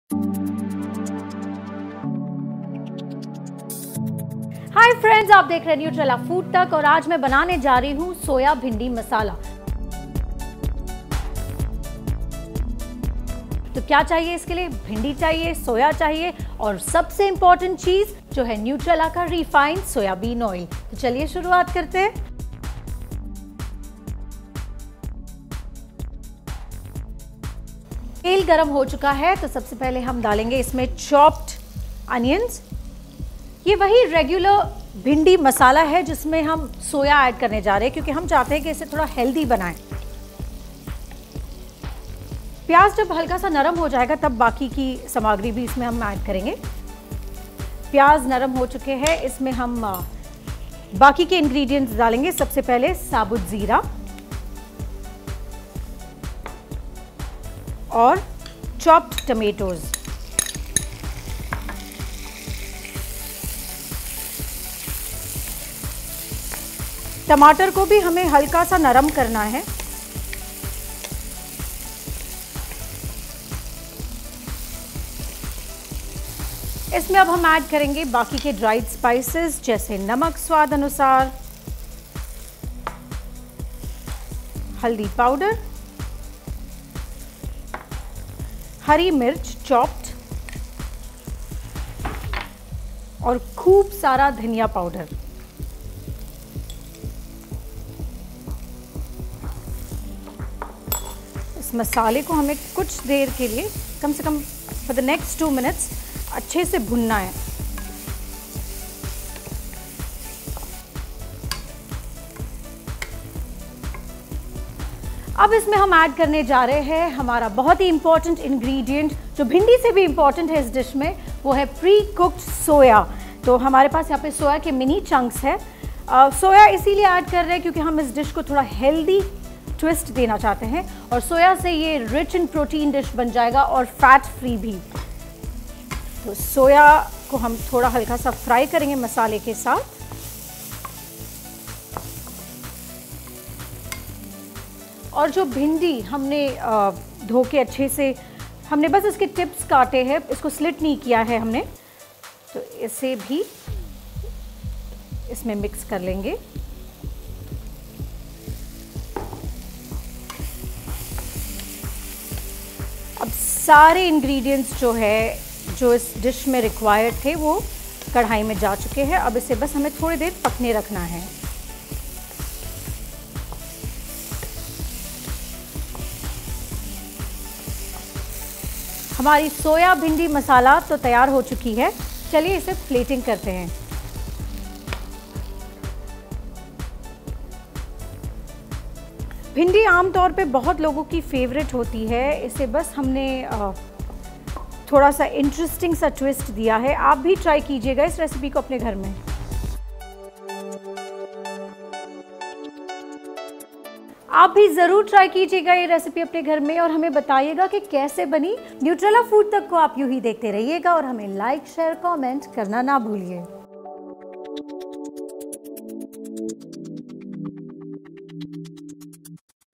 Soya Bhindi Masala Recipe, Lady finger Soya Mini Chunks Recipe - Nutrela
Now make Bhindi Masala protein-rich with this Soya Bhindi Masala Recipe! Watch the video to learn how to make Ladyfinger Soya Mini Chunks Recipe. To get healthy, protein-rich recipes in your inbox, sign up for Madhuri Dixit’s Healthy Recipe Plan,